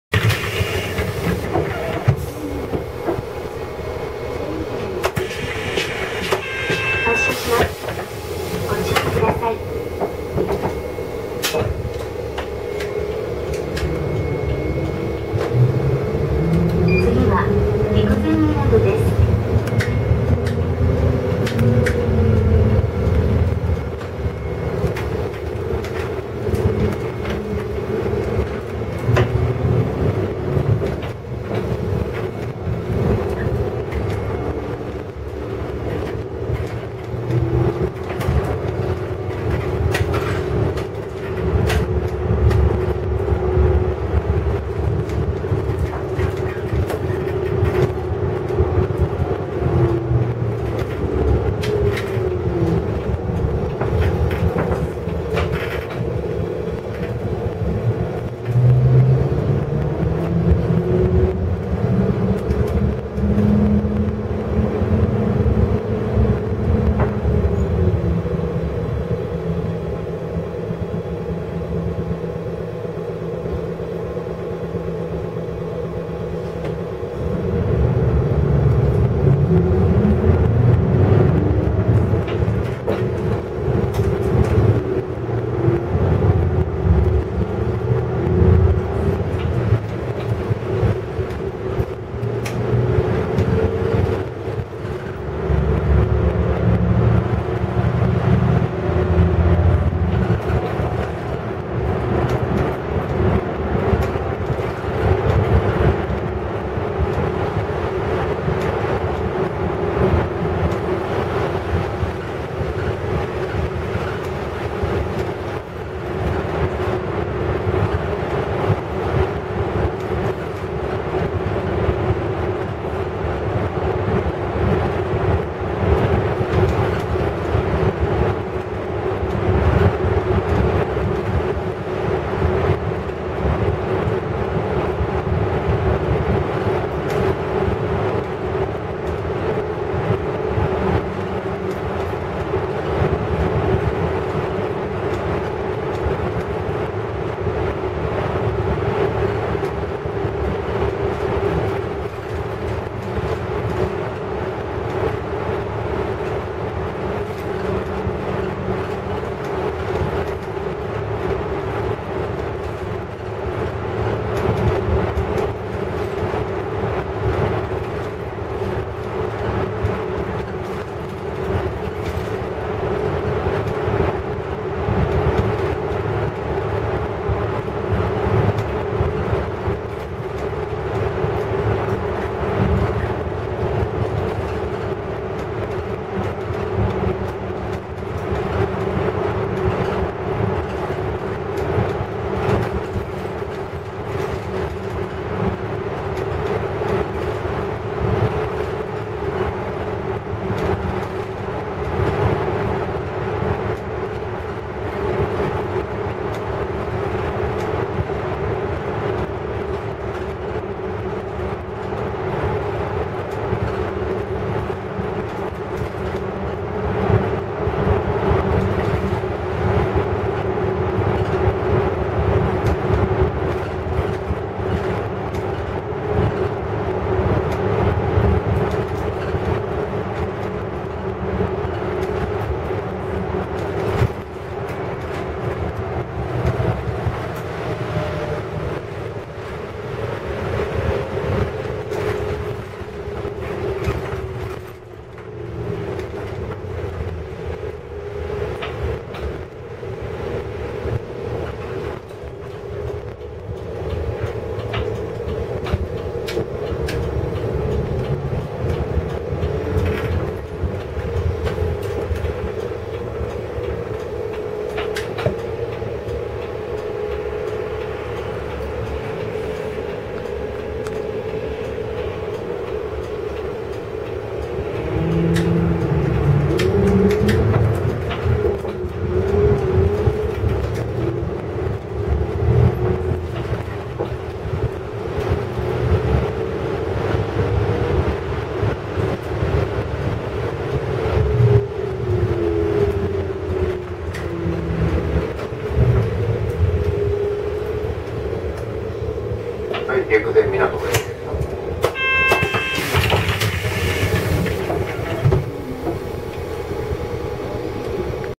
【前面展望】歌津～陸前港間の専用道路を走る気仙沼線BRT
撮影日：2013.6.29　撮影区間：歌津→陸前港
撮影車両：日野・ブルーリボンシティハイブリッドノンステップバス（LNG-HU8JMGP、宮城200か22-20）
トンネル区間を専用道化した区間を走行します。